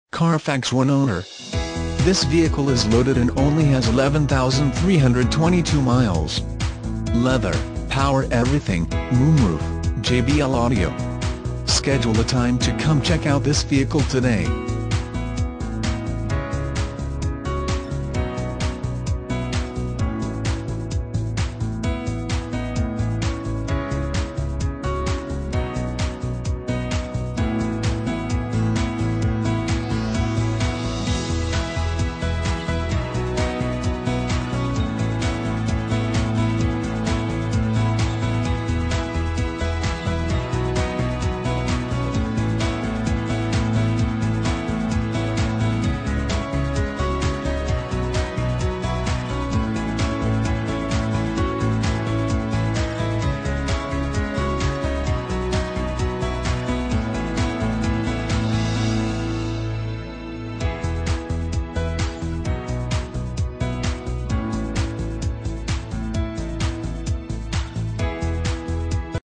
2007 TOYOTA AVALON
2007 TOYOTA AVALON Santa Fe NM

Beaver Toyota
1500 Saint Michaels Dr

Carfax 1-Owner! This vehicle is LOADED and only has 11,322 miles! LEather, Power EVERYTHING, Moon Roof, JBL Audio. Schedule a time to come check out this vehicle today!